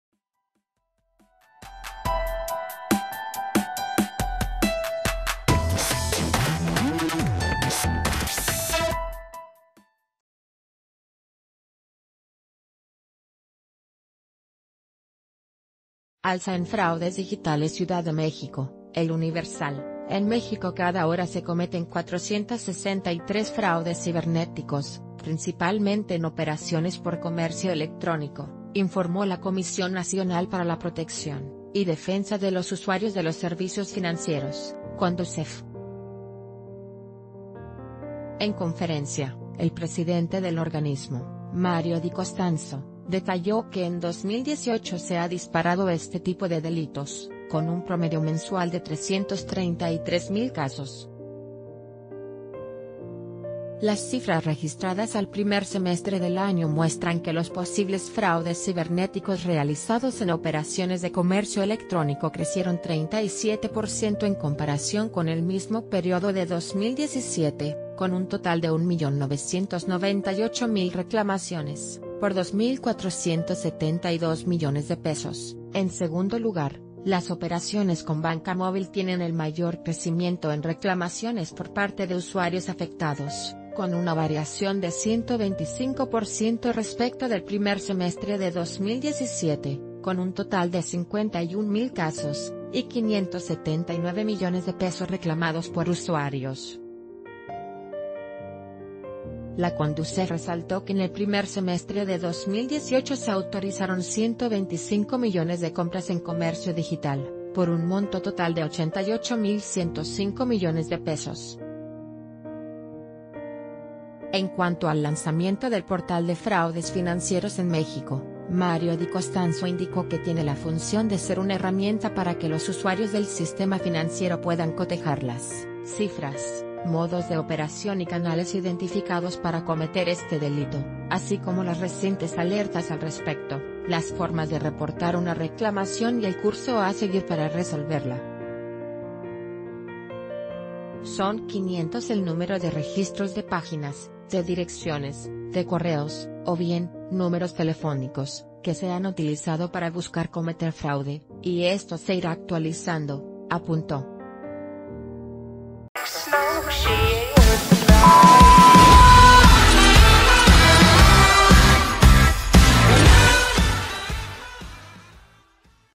La Condusef abre nuevo sitio para hacer denuncias
Alza en fraudes digitales CIUDAD DE MÉXICO (El Universal).— En México cada hora se cometen 463 fraudes cibernéticos, principalmente en operaciones por comercio electrónico, informó la Comisión Nacional para la Protección y Defensa de los Usuarios de los Servicios Financieros (Condusef). En conferenc...